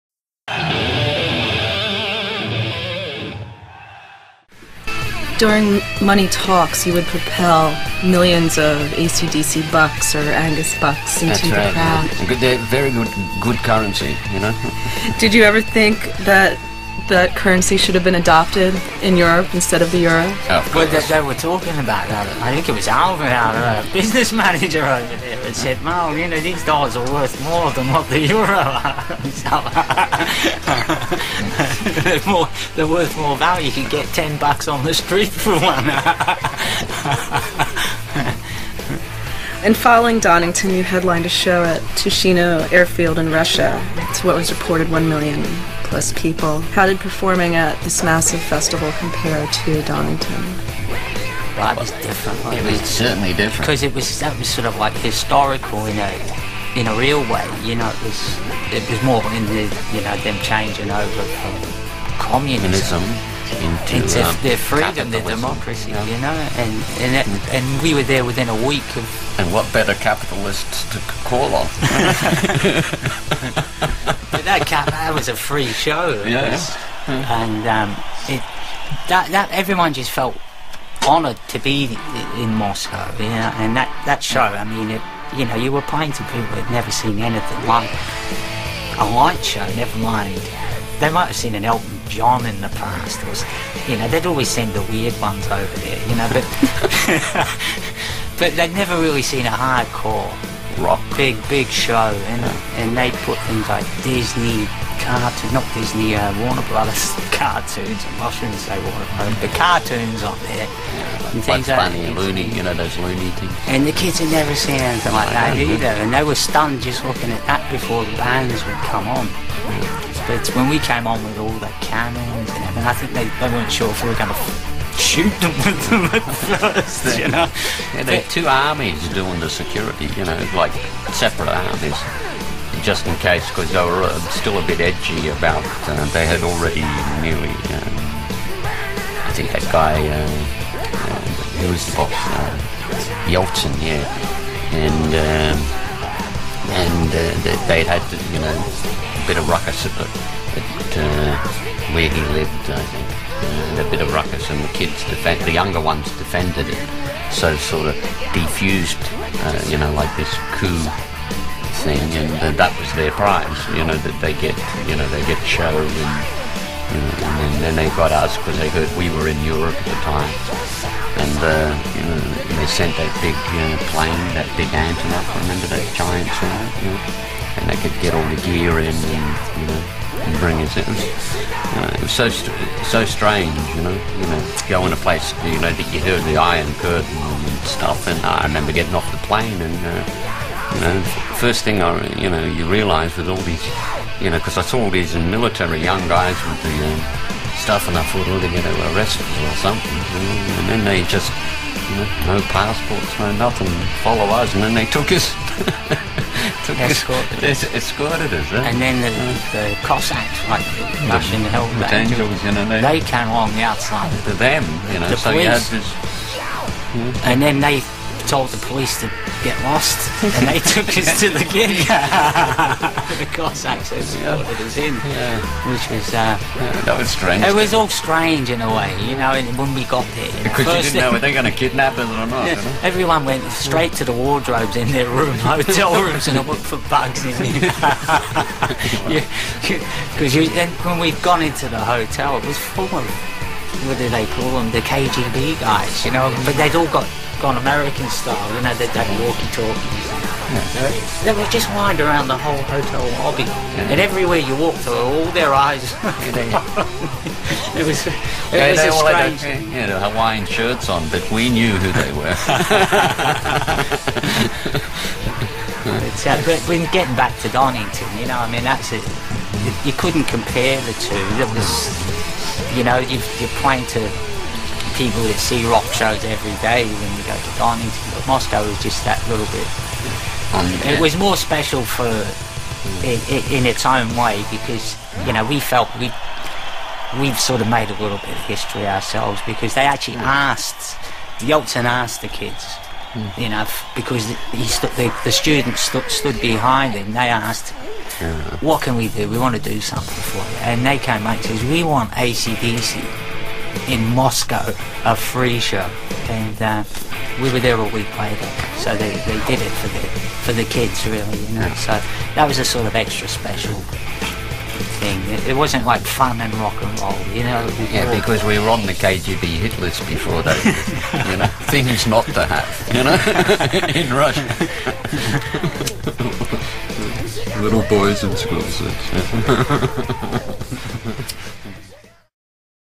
AC/DC LIVE AT DONINGTON The interview (2003) part three.
- Angus Dollar$
- Moscow
- KGB hit list